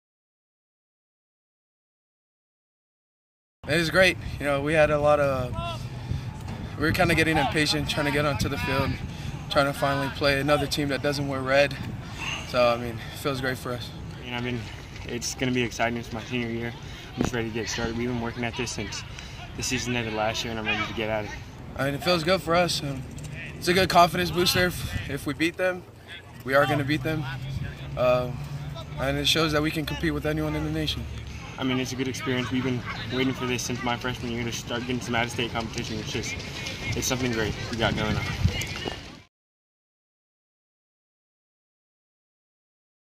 Arbor View prepares for season opener
Arbor  View players discuss their upcoming game against Valley View (California) at Arbor View  High School on Wednesday, Aug. 15, 2018. (Damon Seiters/Las Vegas Review-Journal )